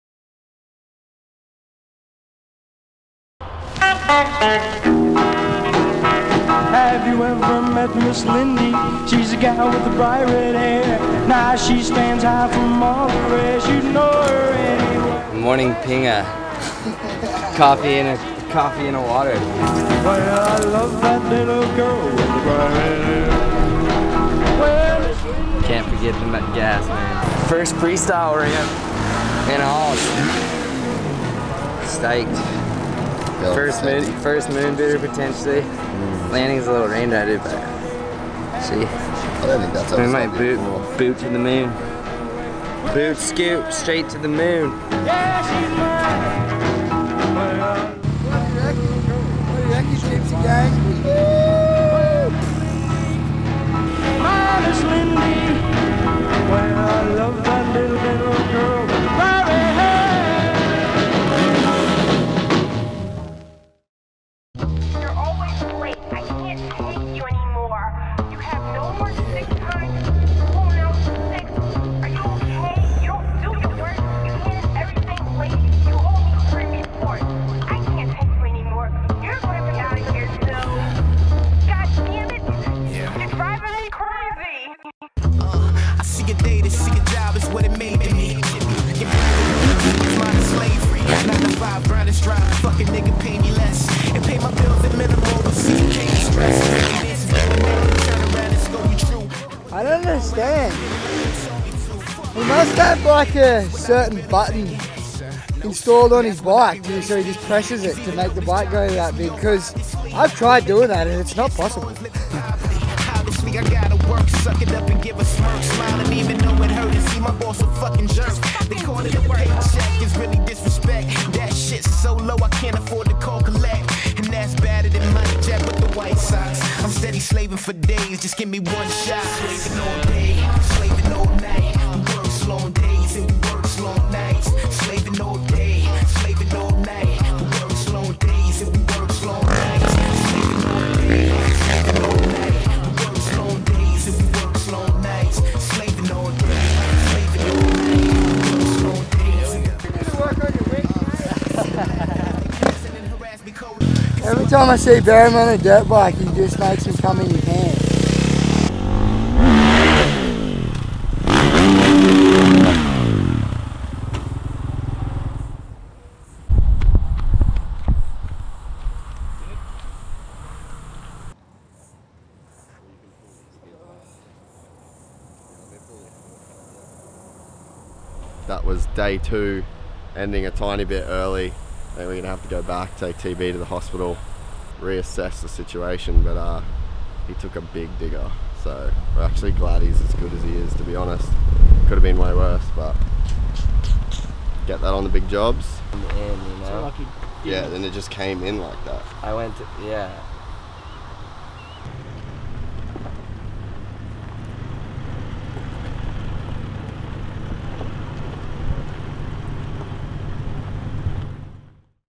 East Bound & Down Under Day 2 - Ft. Tyler Bereman
If you've been following the trip on our Instagram (@gypsytalespodcast) you might have noticed we have been a bit quiet on the posts. Mainly due to the fact that TB took a bit of a digger...

Presented by: FASTHOUSE